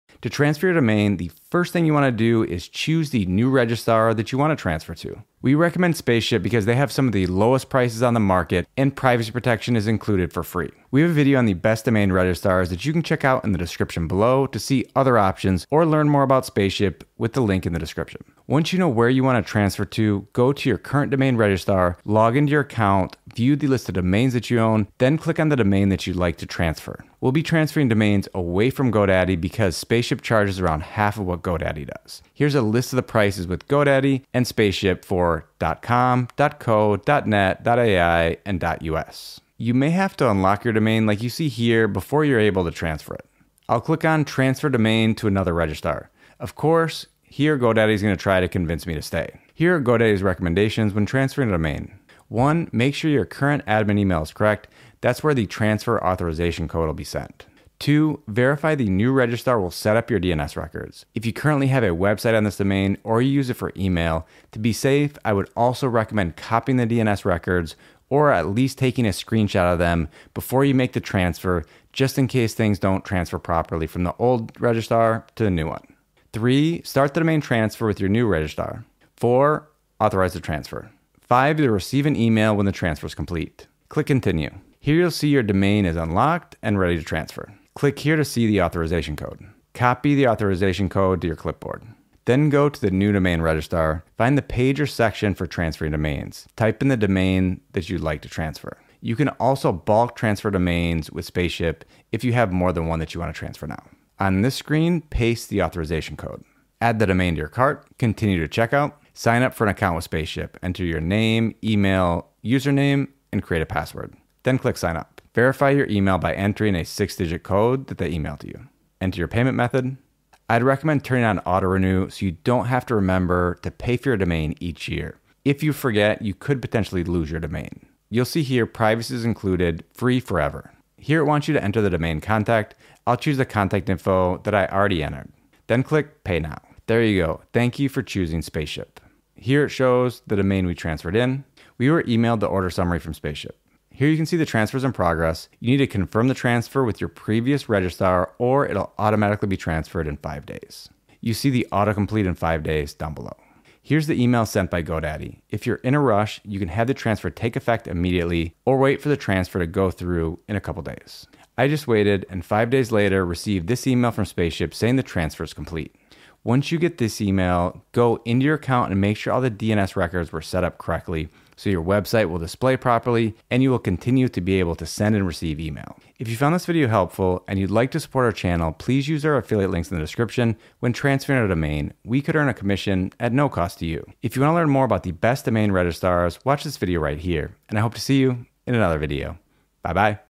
Domain Transfer 2025 - How to Transfer a Domain to Save Money
How to transfer domain in 2025 to save money. If you're with the wrong domain registrar, you could be paying over double what other registrars charge. In this video, I'll walk you through step by step how to transfer your domain to our favorite domain registrar in 2025 Spaceship.